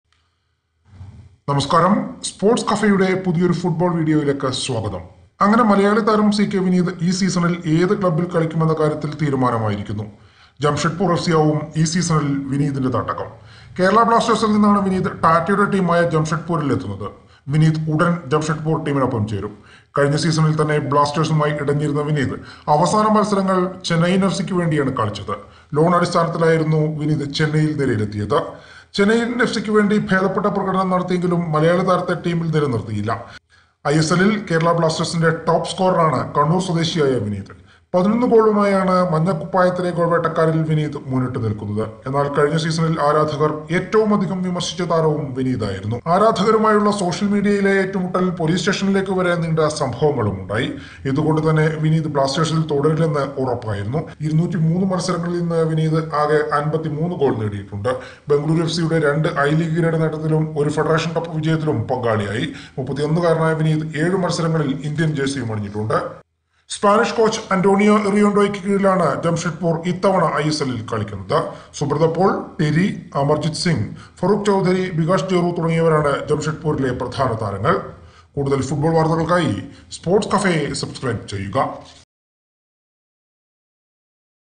Jamshedpur FC Rope in Forward CK Vineeth From Kerala Blasters (Malayalam)
സി കെ വിനീത് കേരള ബ്ലാസ്റ്റേഴ്സ് വിട്ട് ജംഷംഡ്പൂർ എഫ് സിയിൽ
 Jamshedpur FC on Monday added C.K. Vineeth to their squad for the upcoming Indian Super League season.